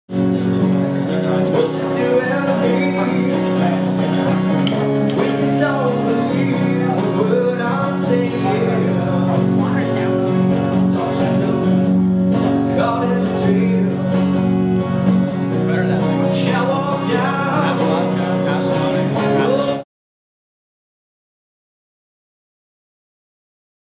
Crab Corner Featuring Free Live Music Entertainment | Seafood Restaurants Las Vegas pt. 31
Crab Corner Featuring Free Live Music Entertainment | Seafood Restaurants Las Vegas
Come join us for Live Music every Wednesday at Crab Corner Southwest Las Vegas between 6-9pm - Enjoy half price bottles of wine, whole lobster specials, and delicious seafood!